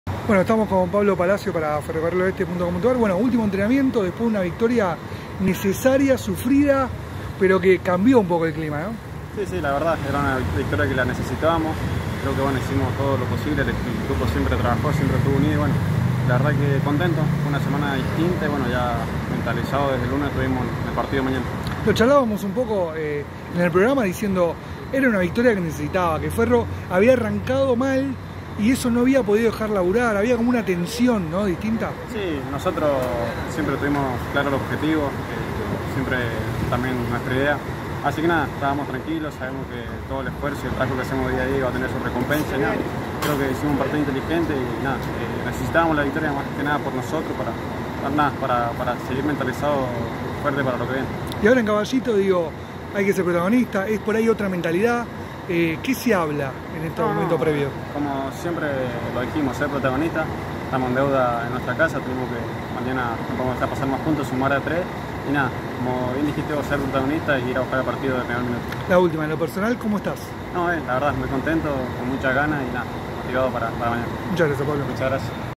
Entrevista a Pablo Palacio
⚽️ En la previa al partido de mañana con DeportivoMadryn charlamos con Pablo Palacio.

16/03/2023